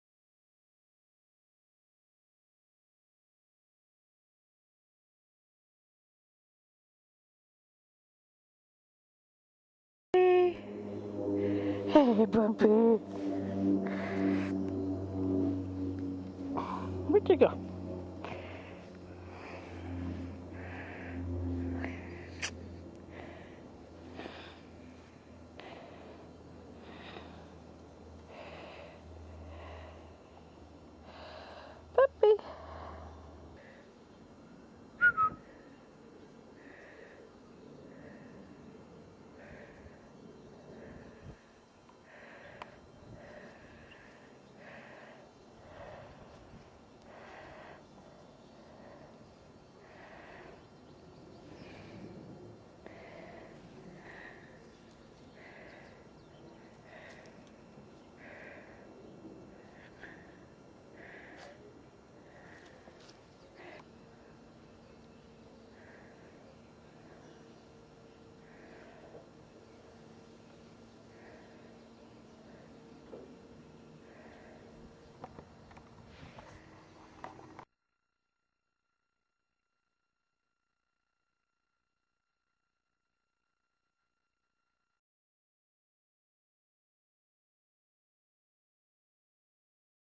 my puppy
this is my puppy when he was a little pup ^_^ isn't he cute
plz rate and comment thankyou. (ps not me talking o.o. ? weird... naah it my mom -.-")